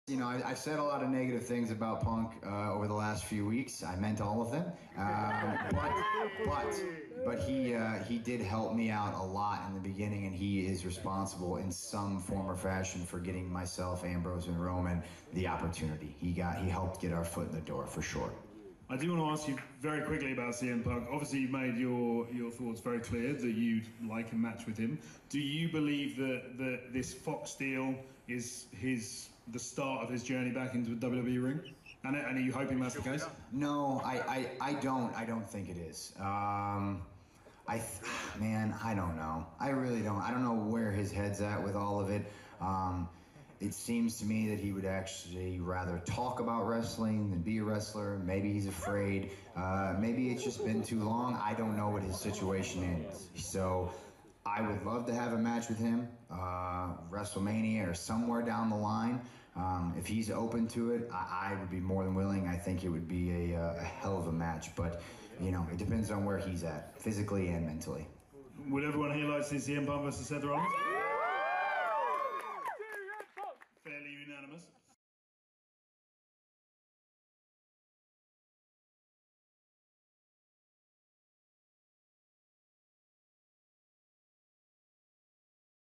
Seth Rollins habla de CM Punk. (Subtitulado en Español.)
Durante una entrevista en 2019 Seth Rollins habló de la situación de CM Punk en cuanto a su regreso a la WWE.